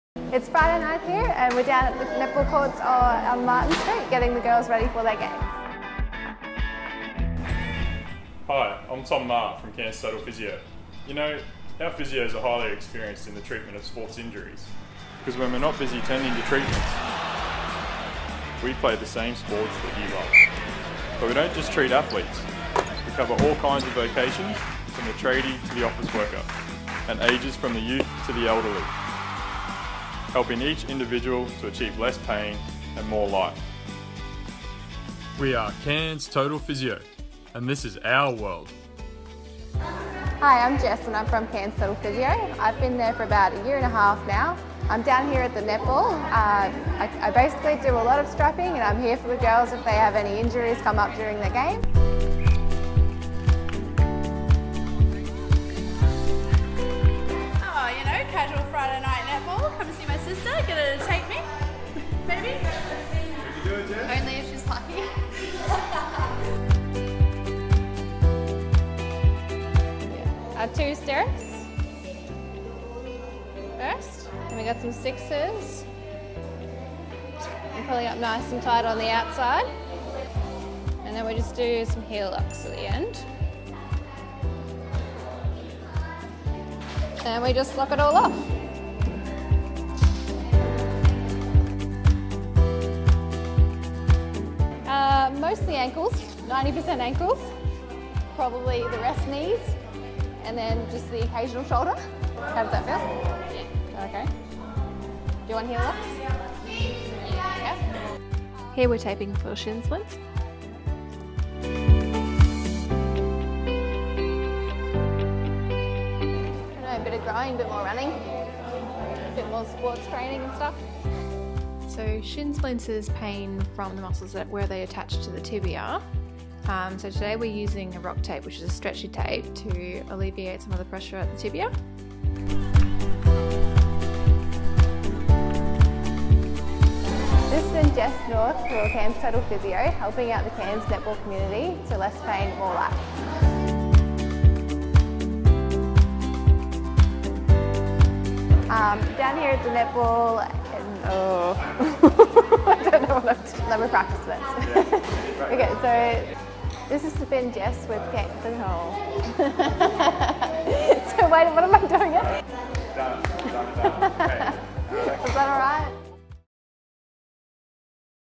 Get To Know The Team - Jess North Physio
We follow Jess down to the Cairns Netball Association as she prepares the players for their weekly matches on a Friday night.